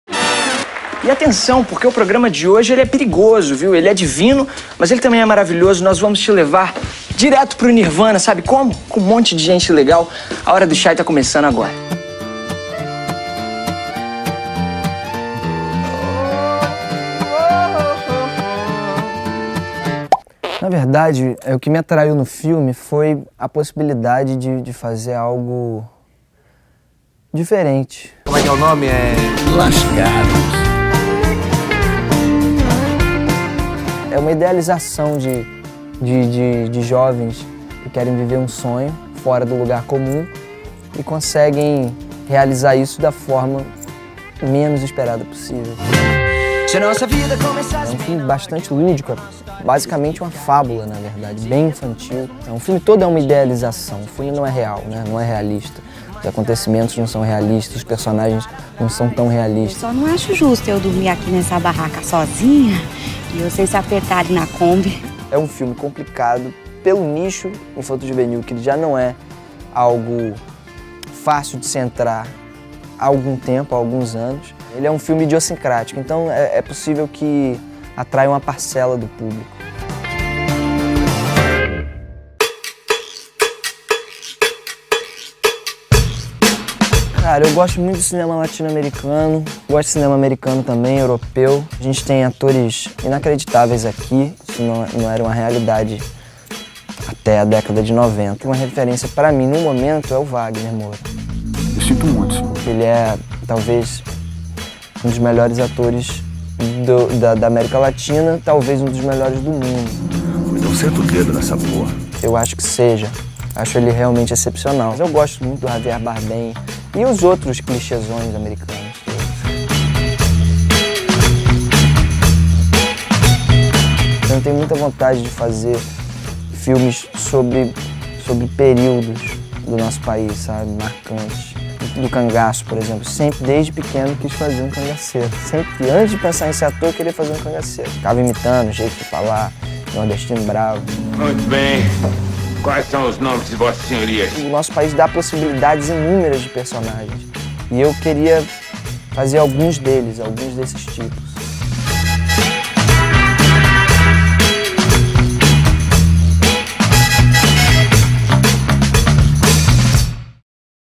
Chay Suede e sua aposta infantojuvenil: o filme "Lascados"
É ao volante de uma Kombi velha que Chay Suede, 22, espera recuperar um público perdido: o infantojuvenil, que anda afastado dos filmes nacionais nos últimos tempos.

O galã global protagoniza "Lascados", longa que estreou na última quinta (18), de olho em crianças e adolescentes. No filme —um "road movie teen" dirigido pelo estreante Vitor Mafra—, Suede interpreta um estudante paulista que viaja com dois amigos para o Carnaval de Salvador a bordo de uma Kombi.

Reportagem: Guilherme Genestreti

Fotografia: Caros Cecconello

Edição: Giovanni Bello